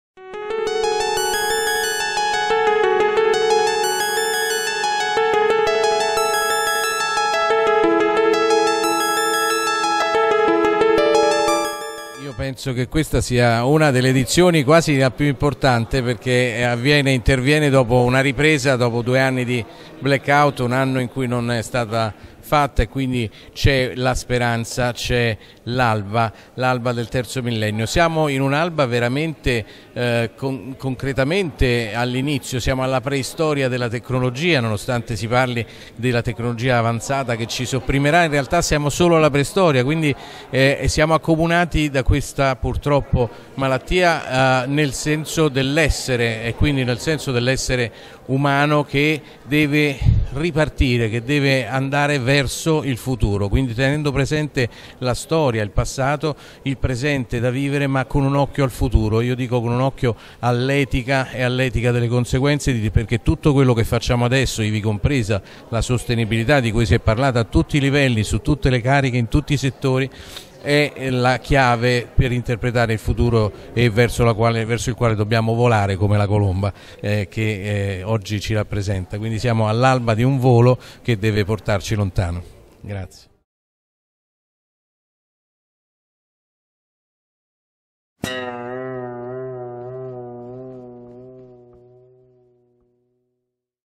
Stefano Crisci - Premio Le Ragioni della Nuova Politica XVIII ed.
Stefano Crisci, Avvocato e Docente universitario - Premio Le Ragioni della Nuova Politica XVIII edizione organizzato dall'Associazione culturale L'ALBA DEL TERZO MILLENNIO - Sala Vanvitelli dell'Avvocatura Generale dello Stato - 15 dicembre 2021 - A cura di Gianni Colacione